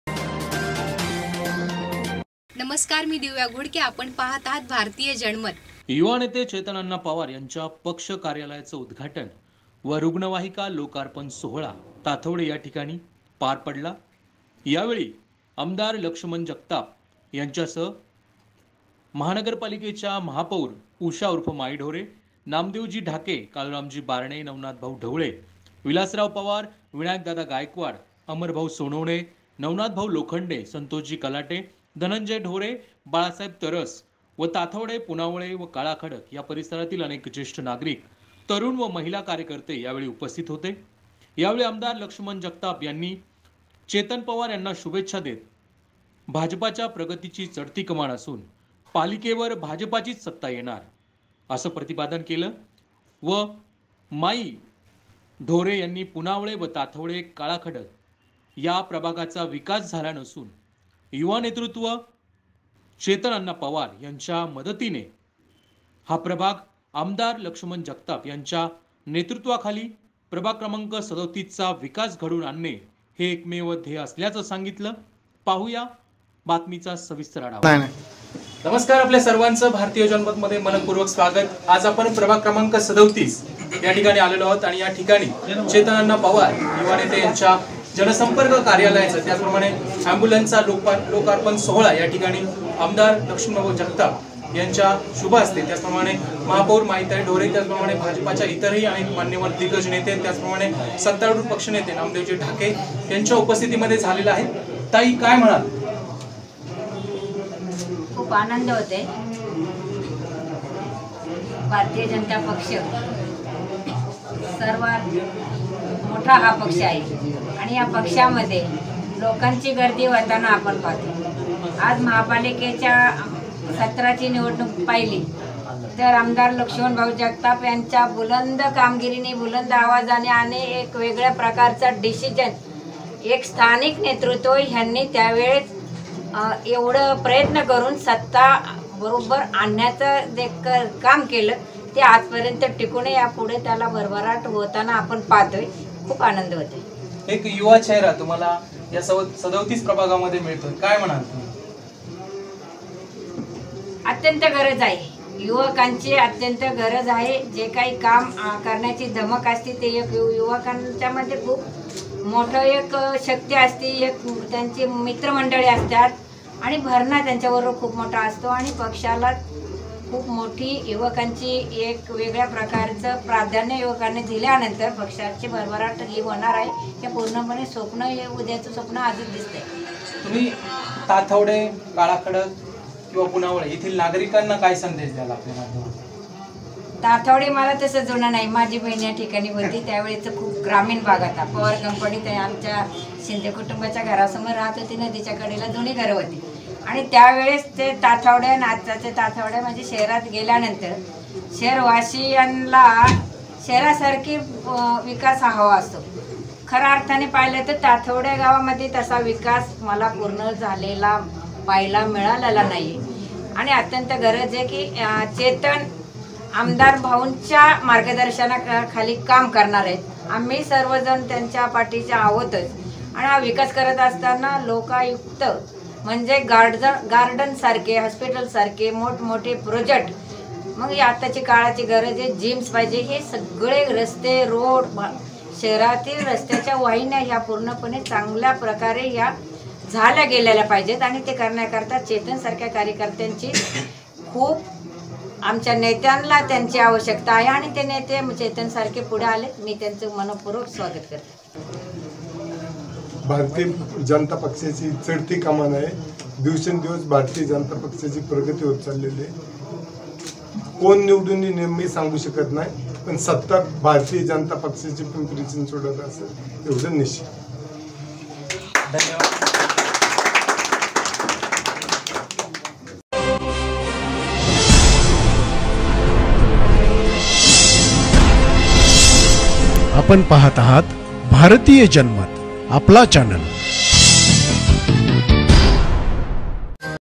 पिं चीं मनपा निवडणुक मिशन २०२२ सत्ता भाजपाचीच येणार -आमदार लक्ष्मणभाऊ जगताप..........
हमें ट्विटर पर फॉलो करें : Bhartiya Janmat